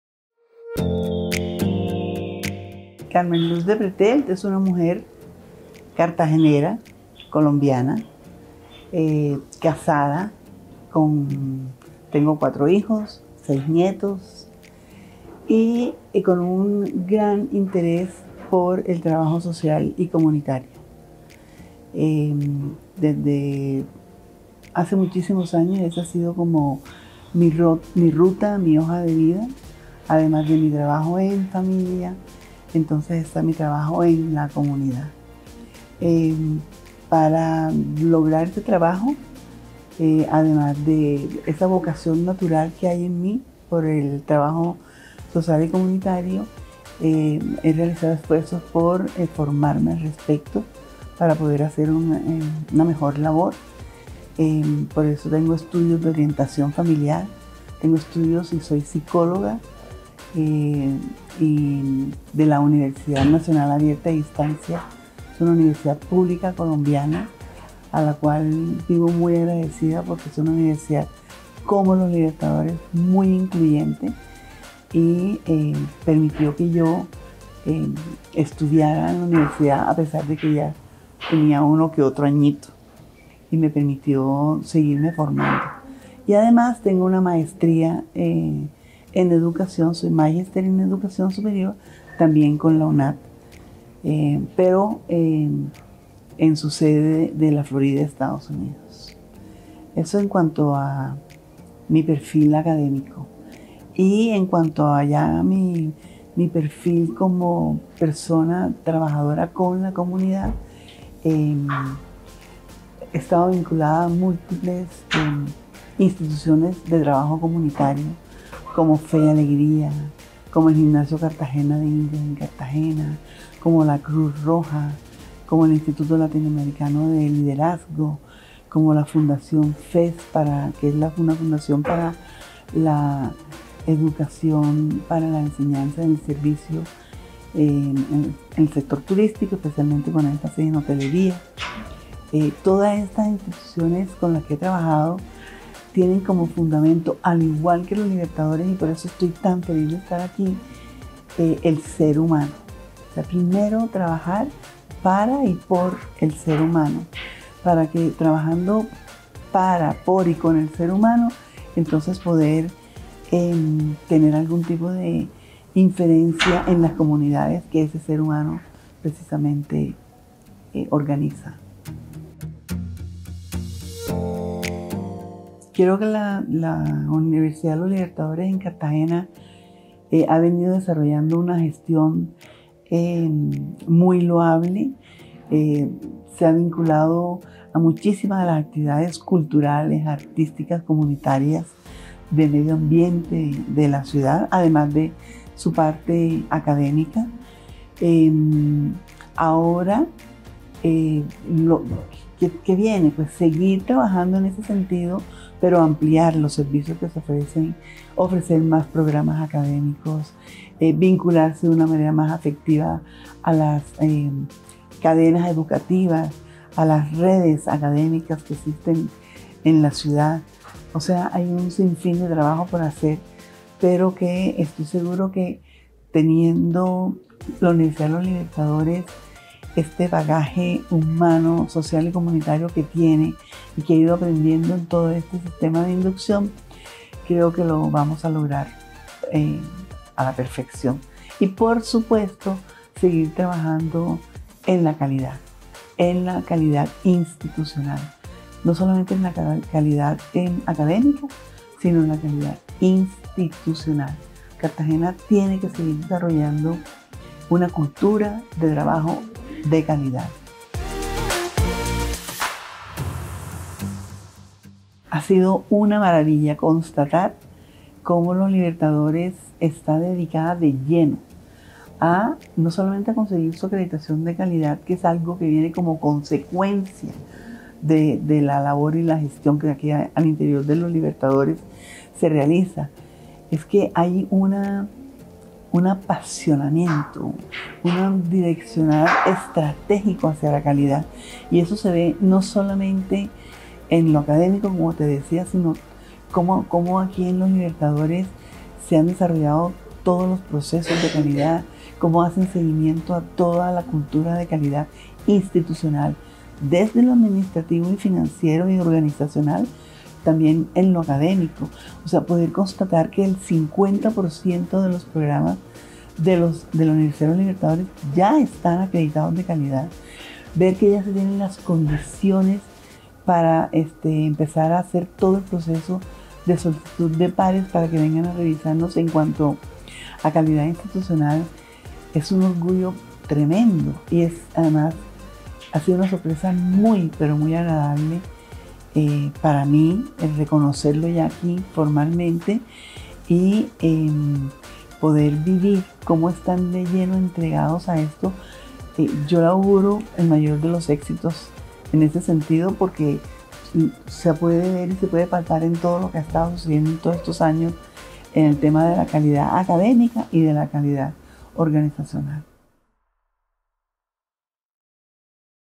Carmen Luz De Pretelt nueva Directora FULL Cartagena
Nuestra nueva directora de FULL Cartagena Carmen Luz De Pretelt nos cuenta un poco de lo que serán los nuevos retos y metas de la Institución.